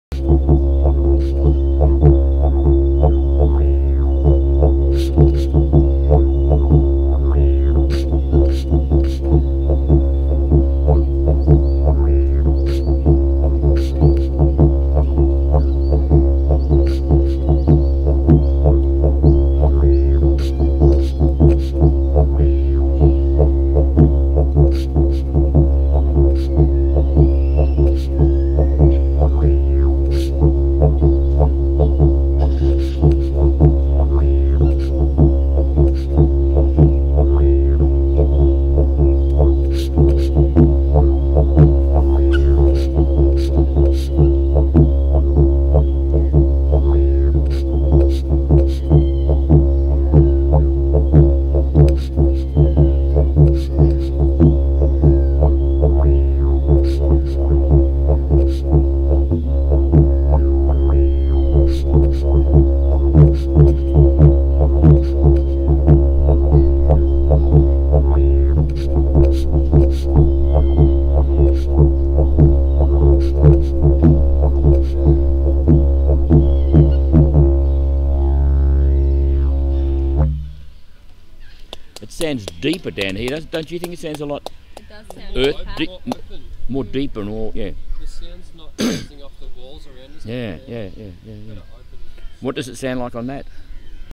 Murrundindi playing the Didge
Murrundindi the leader of the Wurundjeri people playing the Didge at healsville sanctuary.
2016.